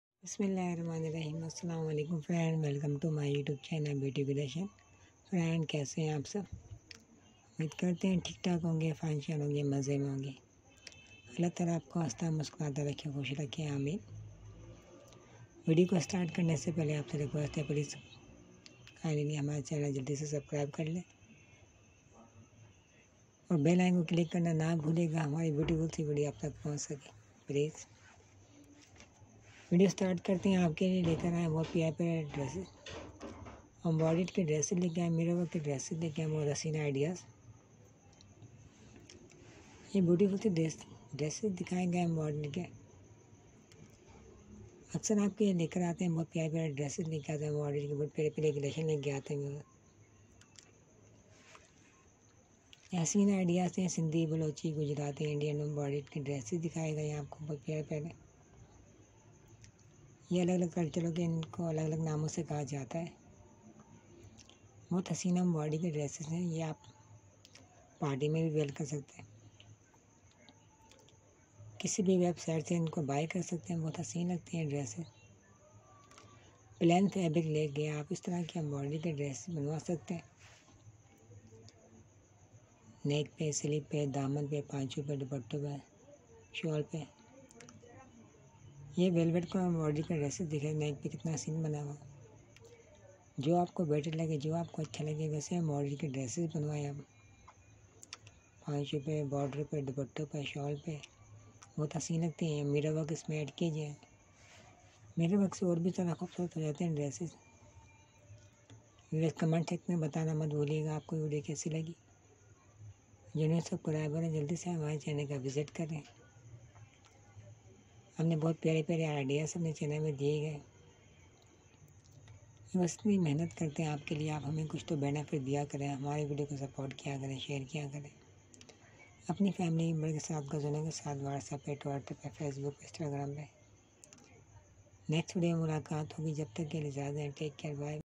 Mirror work with traditional Sindhi balochi Gujarati hand embroidery designs
is video me ap dakhe ge Mirror work with traditional Sindhi balochi Gujarati hand embroidery designs